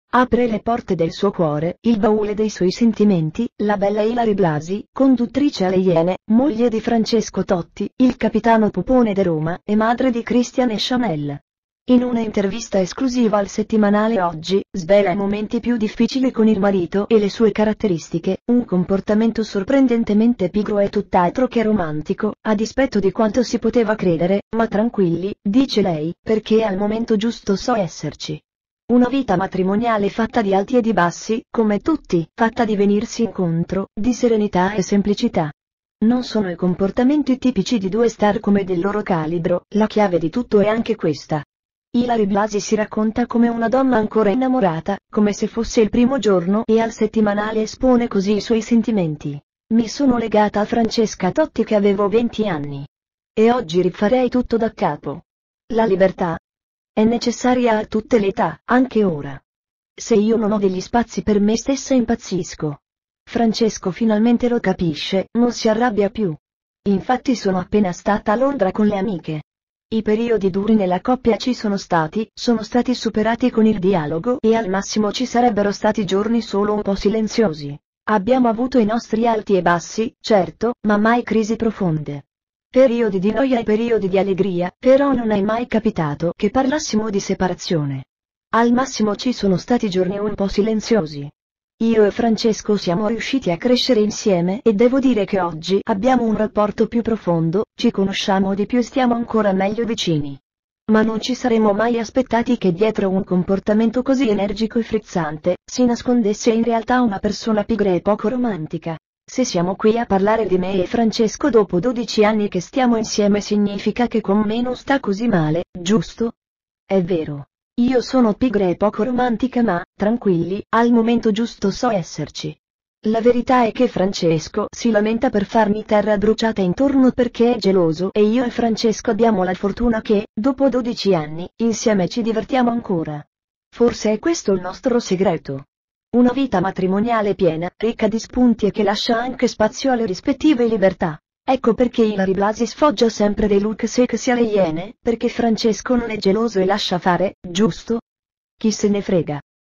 Ilary Blasi “Con Totti tanti alti e bassi, ma adesso ho le mie libertà”
La conduttrice de "Le Iene" si racconta in un'intervista esclusiva a "Oggi", dalle sue aspettative al suo rapporto con il "Pupone de Roma", Francesco Totti.